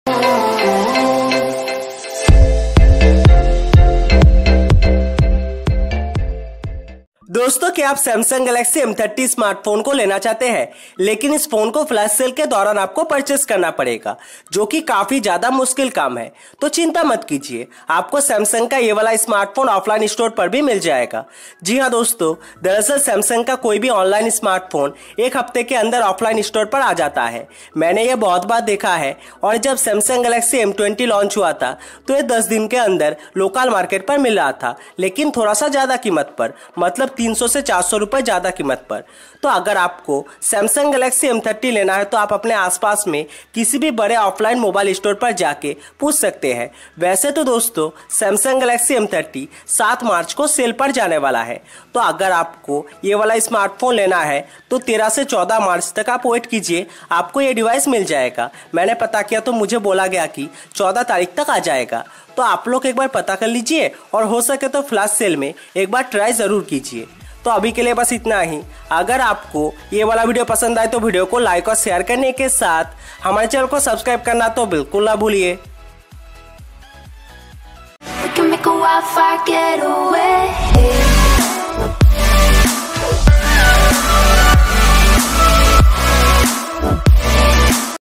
Samsung Galaxy M30 Will Available In Offline Store In India
Welcome To APP2USE Youtube Channel.

Friends In This Video I Will Tell You Some Information About Samsung Galaxy M30 Offline Availability In India.